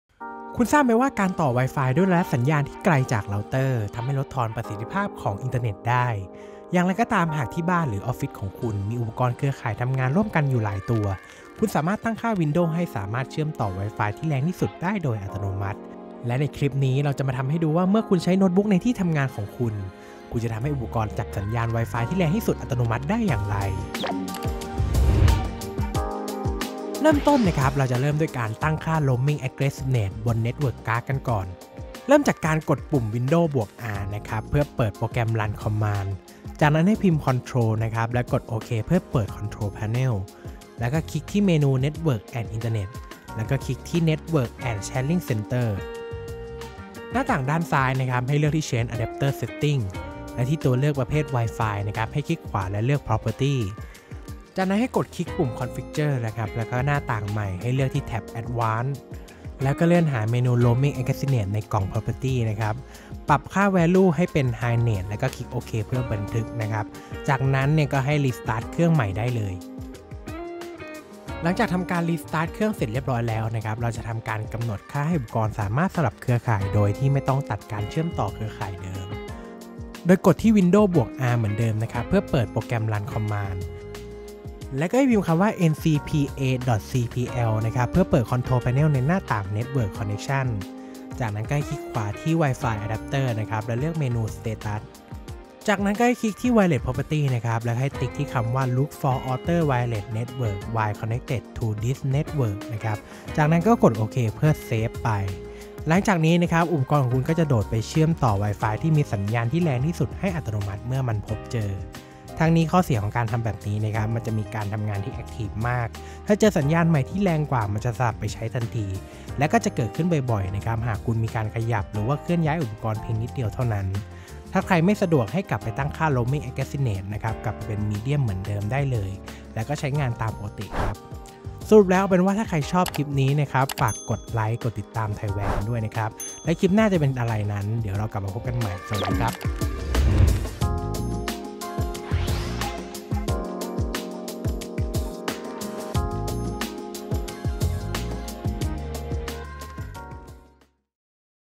วิธีตั้งค่าโน้ตบุ๊คให้สลับ Wi-Fi ที่สัญญาณแรงสุดอัตโนมัติ
หากในบ้านหรือออฟฟิศคุณมีอุปกรณ์ Wi-Fi หลายตัว คลิปนี้เราจะมาสอนวิธีให้คุณสามารถใช้งานระบบ Wi-Fi ในบ้านได้สะดวกมากขึ้นด้วยการตั้งค่าให้มันสลับใช้ Wi-Fi อัตโนมัติเมื่อเจอสัญญาณที่แรงกว่า

► อ่านทิปส์ และ ทริคของ Thaiware อื่น ๆ เพิ่มเติมได้ที่นี่